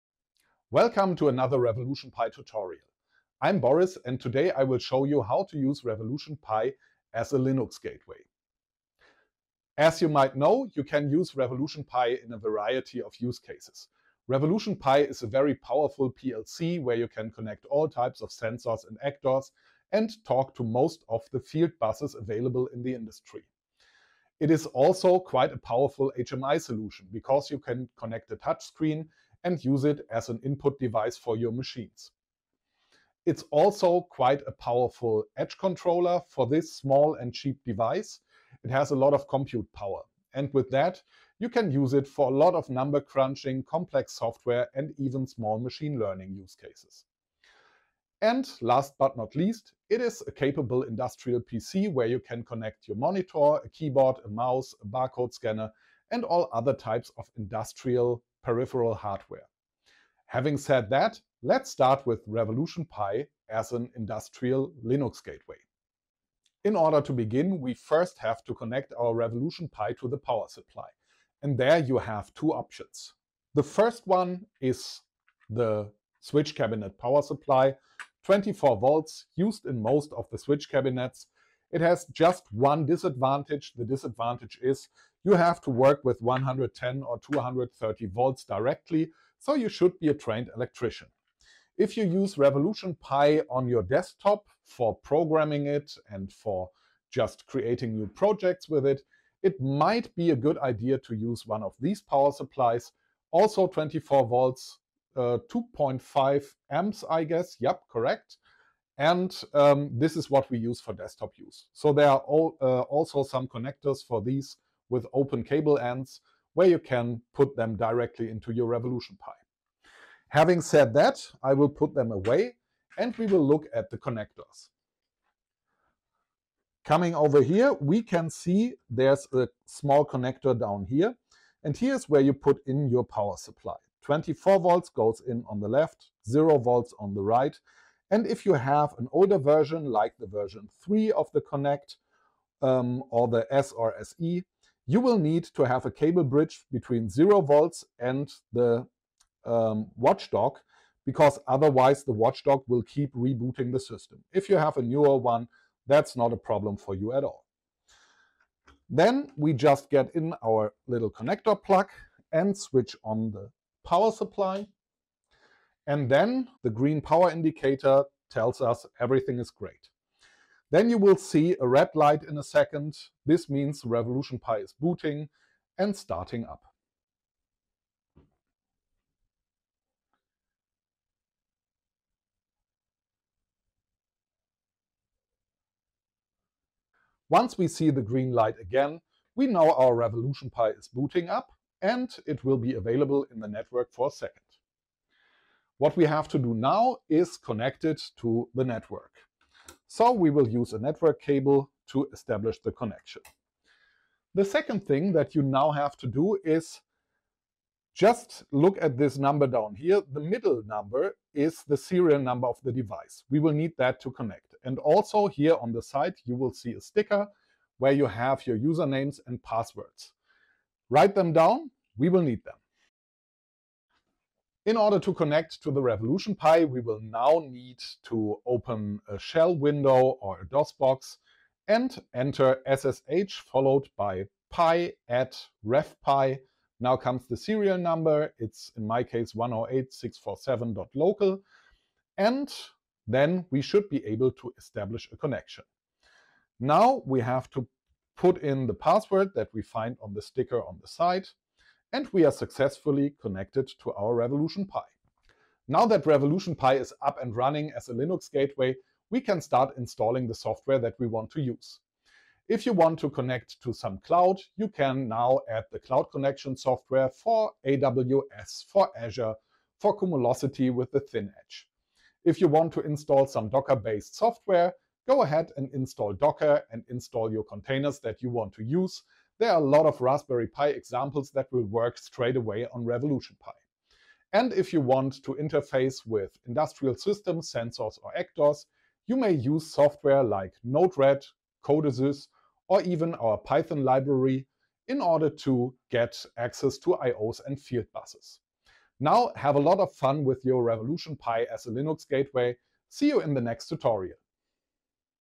Revolution Pi - the industrial Raspberry Pi - as Linux Gateway
Revolution Pi as Linux Gateway

✅ Wiring up your RevPi
✅ Log on to the Raspberry Pi OS

Possible next steps:

// CLOUD Connections

// PLC Programming